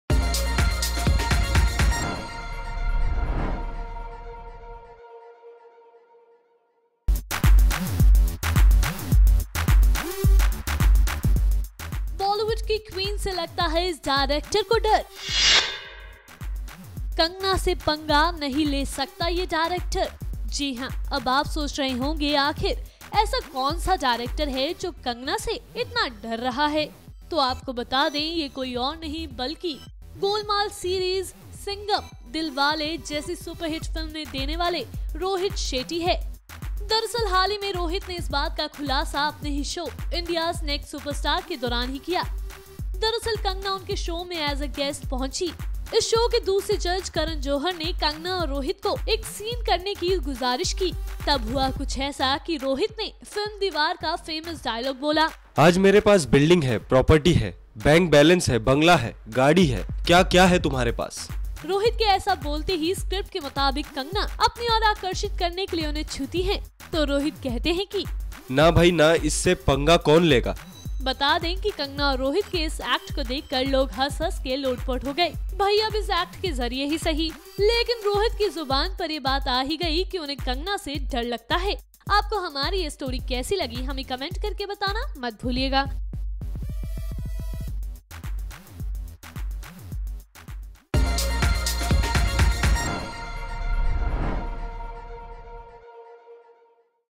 kangana से पंगा लेने से डरते है rohit, दिया बड़ा बयान
Filmmaker Rohit Shetty is in no mood to mess with actress Kangana Ranaut. The director expressed his views while shooting for TV show "India's Next Superstars".